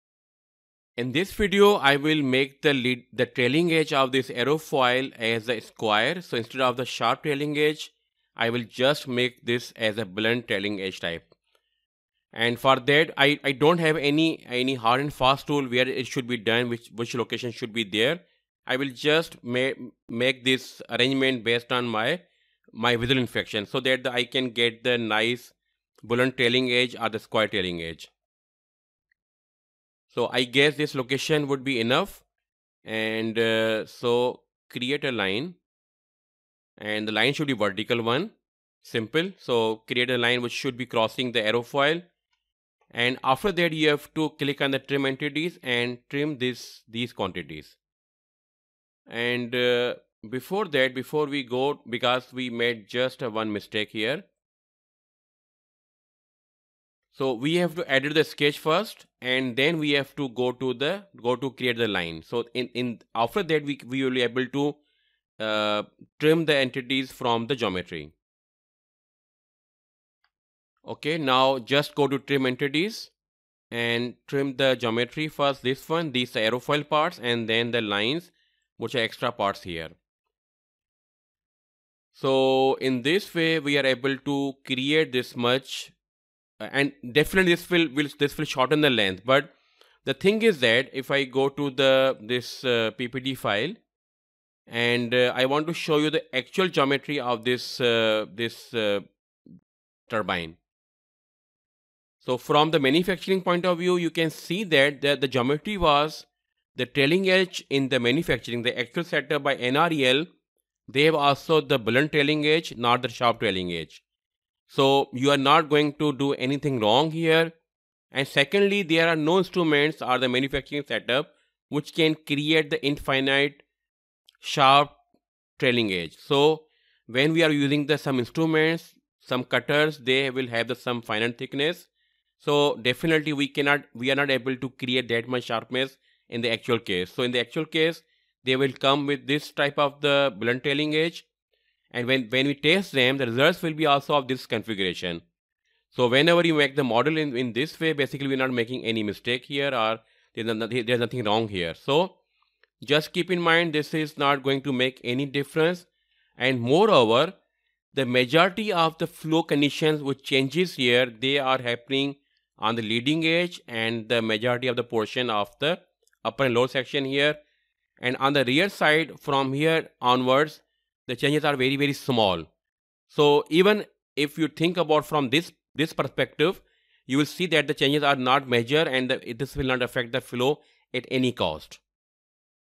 Part 6 - Creating square trailing edge
In this course you will learn about geometry modeling of NREL Phase VI horizontal axis wind turbine in SOLIDWORKS. This method is applicable on all wind turbine solid modeling in solidworks.